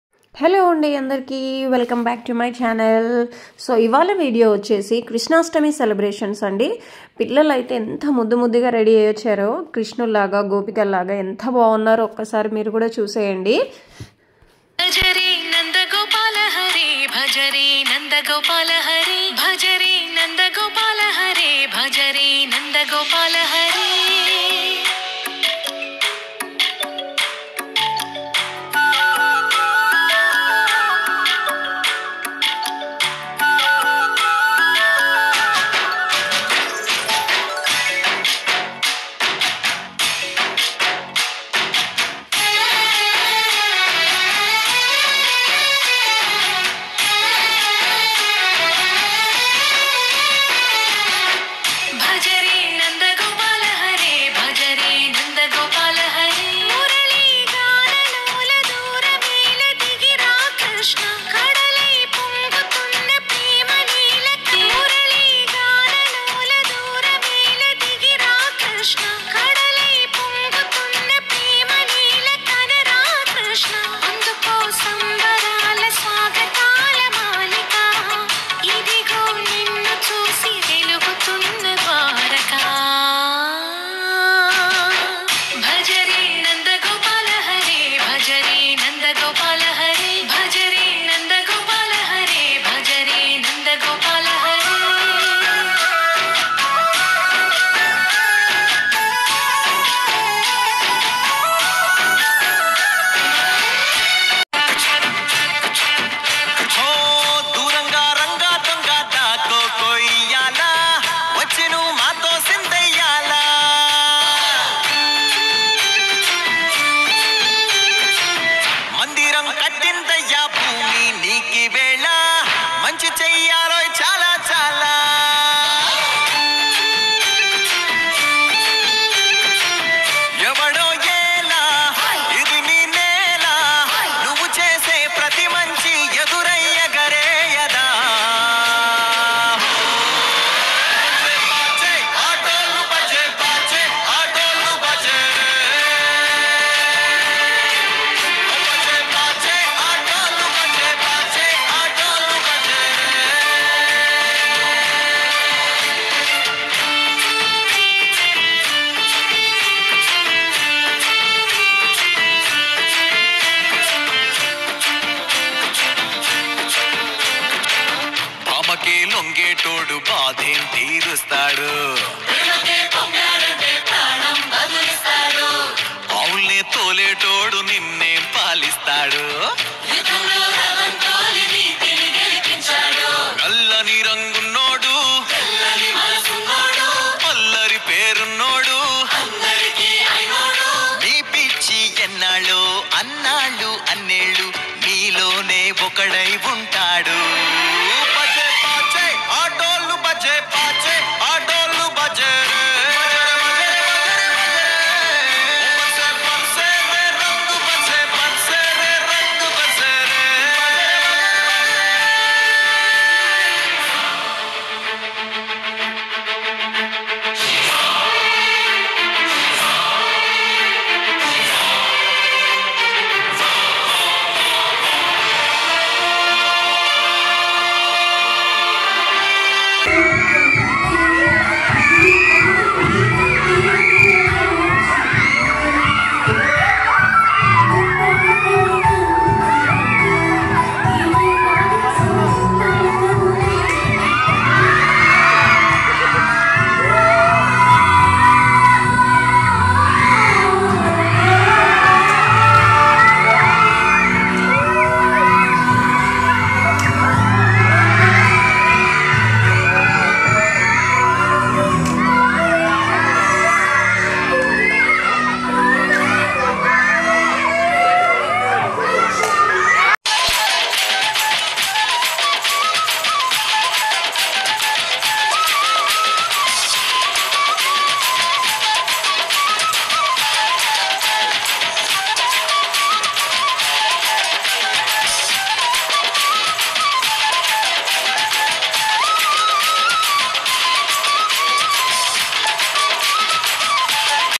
Krishnashtami Celebrations at school 🏫Bodhi Vriksha Olympiad School Kolatam Utti🤩🥳
Bodhi vriksha Olympiad school 🏫subash Nagar